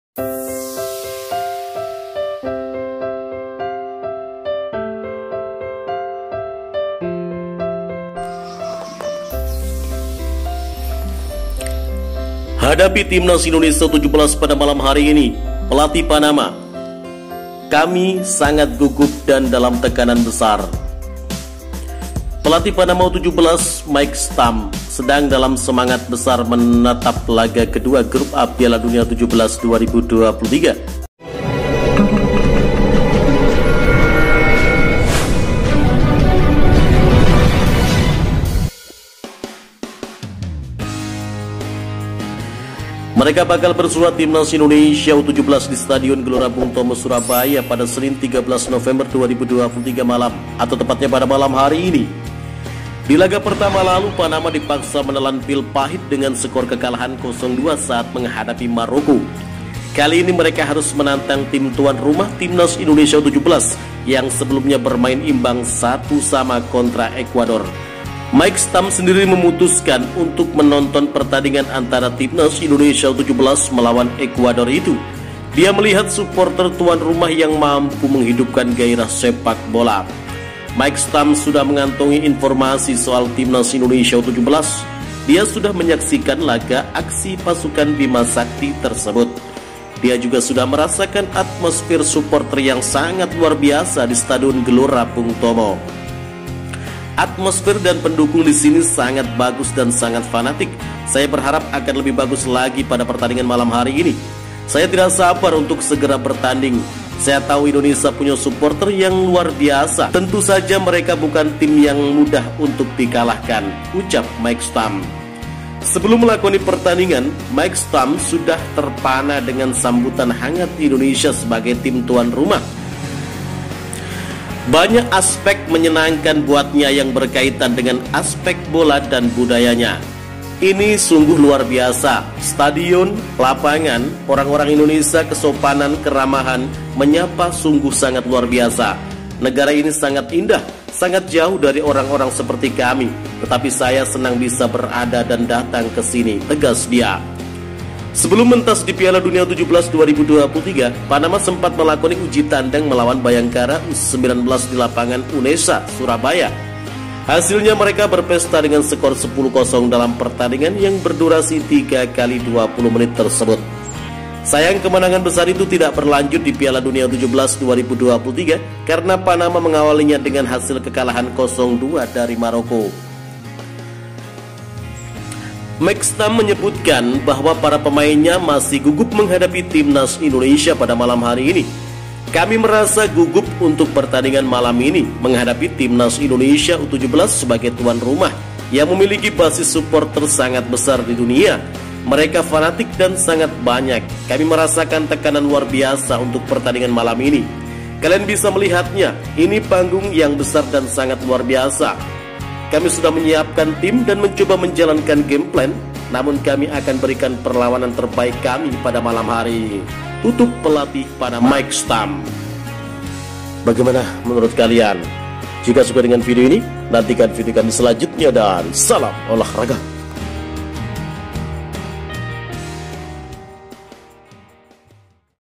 Pelatih Panama Akui Gugup dan Demam panggung hadapi Timnas U17,Efek Stadion dan Suporter ~luar biasa
TimnasIndonesiaVSpanama PelatihPanama StadionGBTsurabaya PialaDuniaU17-2023